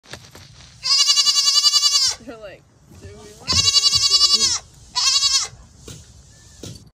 Cute Baby Goats with No Ears
Watch this cute animal video of these cute baby goats jumping on the homestead.